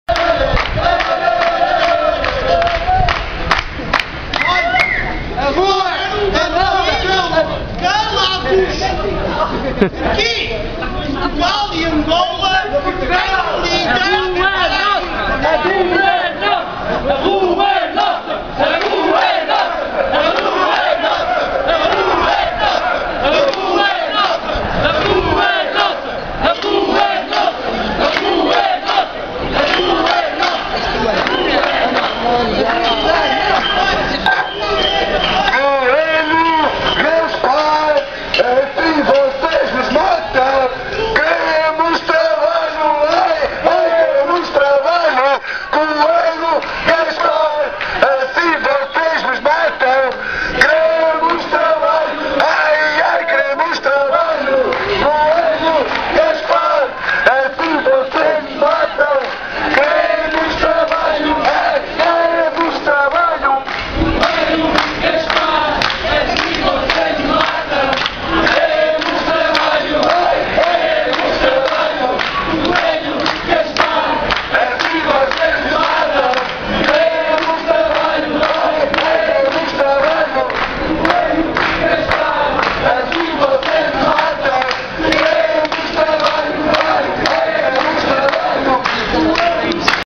Manif. pelo Direito ao Emprego: "Coelho, Gaspar, assim vocês nos matam"
Manifestação pelo Direito ao Trabalho organizad apelo MSE (Movimento Sem Emprego).